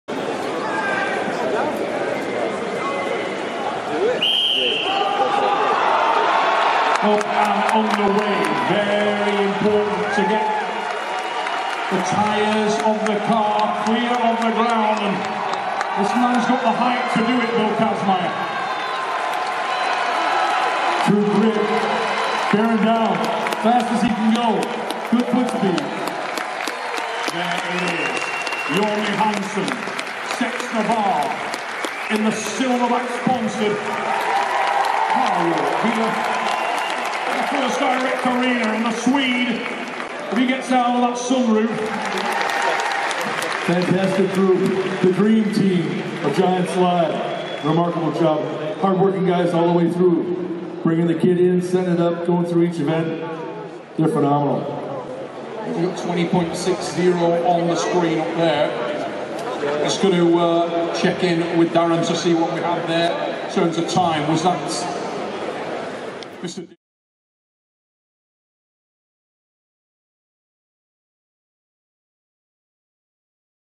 Johnny Hansson | Car Walk | Europe's Strongest Man 2021
Johnny Hansson goes it alone, in the car walk, at Europe's Strongest Man 2021.